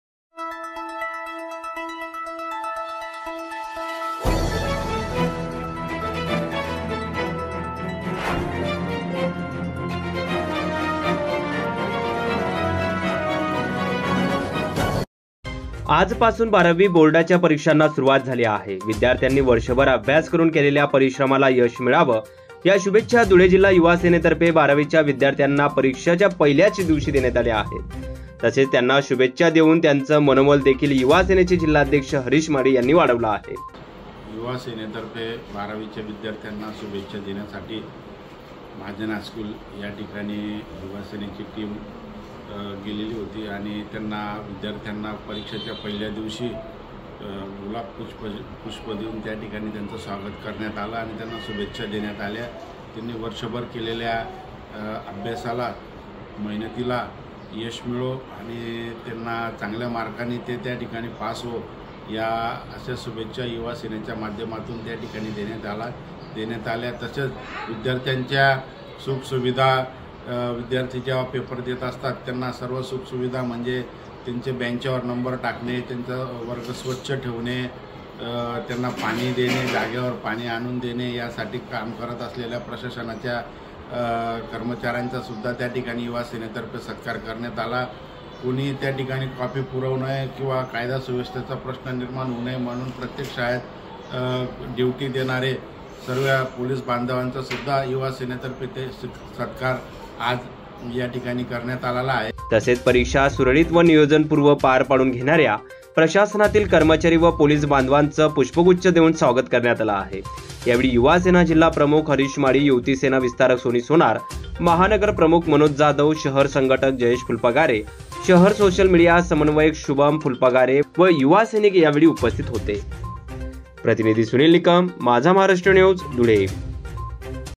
12वी च्या विद्यार्थ्यांना परीक्षेच्या पहिल्या दिवशी पुष्पगुच्छ देऊन शुभेच्छा.
आज पासून बारावी बोर्डाच्या परीक्षांना सुरुवात झाली असून विद्यार्थ्यांनी वर्षभर अभ्यास करून केलेल्या परिश्रमाला यश मिळावे या शुभेच्छा धुळे जिल्हा युवासेनेतर्फे बारावीच्या  विद्यार्थ्यांना परीक्षेच्या पहिल्या दिवशी दिल्या आहेत.  तसेच त्यांना शुभेच्छा देऊन त्यांचं मनोबल युवा सेनेचे जिल्हाध्यक्ष हरिष माळी यांनी वाढवला आहे.